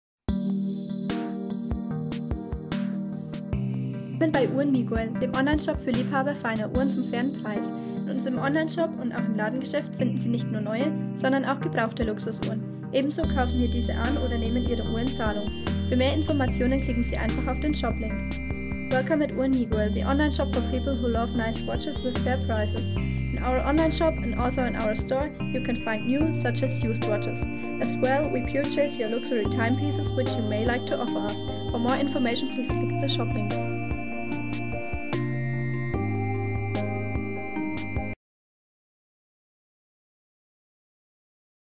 IWC Pilot's Watch Mark XVIII Ref. IW327001 FM12272
IWC Pilot's Watch Mark XVIII

Referenz:
Ref. IW327001
Geschlecht:
Herrenuhr
Gehäuse:
Edelstahl, Gehäusemaße 40 mm, Höhe ca. 10,8 mm, wasserdicht bis 6 atm, Saphirglas, verschraubter Boden und Krone